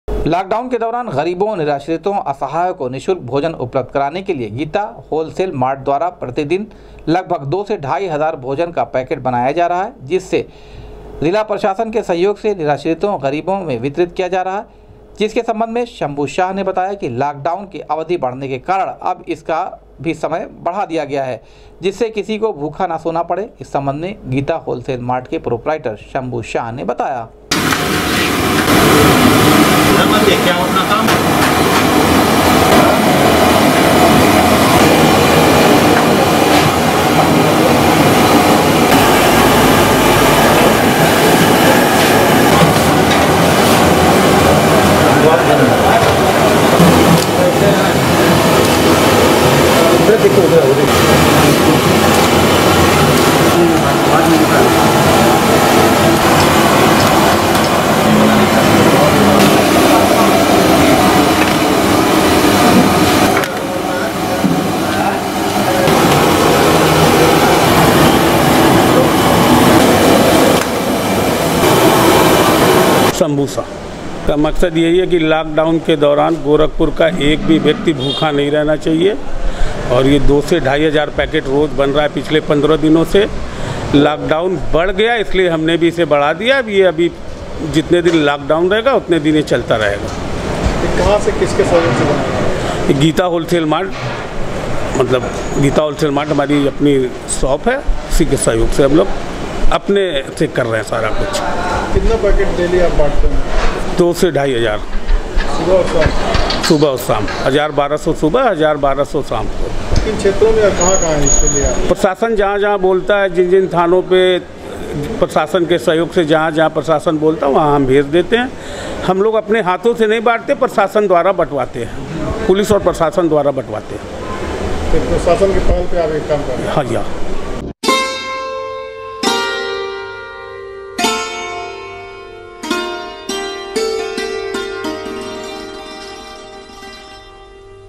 गरीबों निराश्रितों असहायों को निःशुल्क भोजन उपलब्ध करा रहा है गीता होलसेल मार्ट,
लॉक डाउन के दौरान गरीबों निराश्रितों असहायों को निःशुल्क भोजन उपलब्ध करा रहा है गीता होलसेल मार्ट, जिला प्रशासन के द्वारा गरीबो में हो रहा है वितरण।।।। GORAKHPUR NEWS -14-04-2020